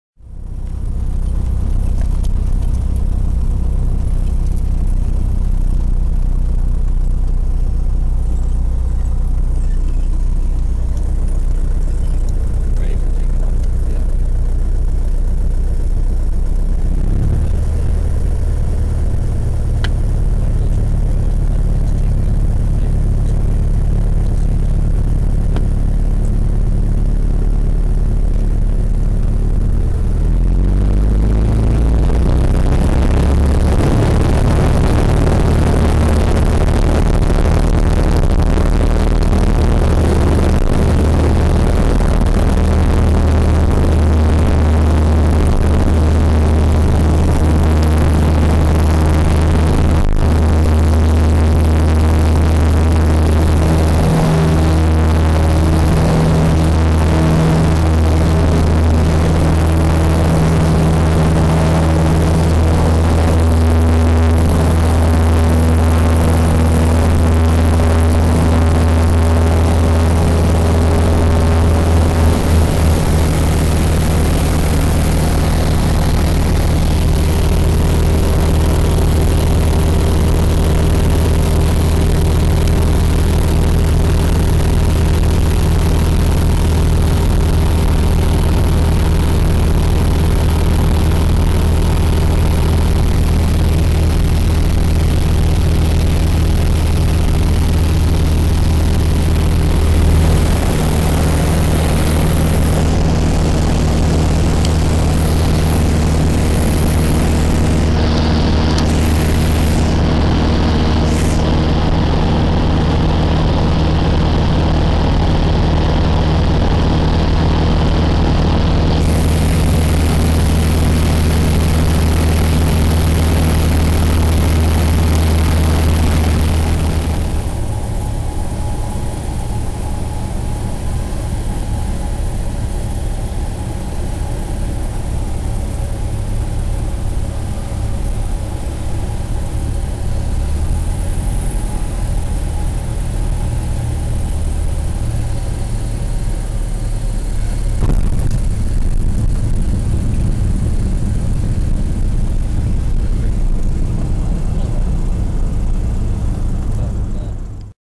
Flight from Waterville (CCW3) to St. John (CYSJ)
Just a few clips from a flight from Waterville, NS, to Saint John, NB.  It was a fun flight!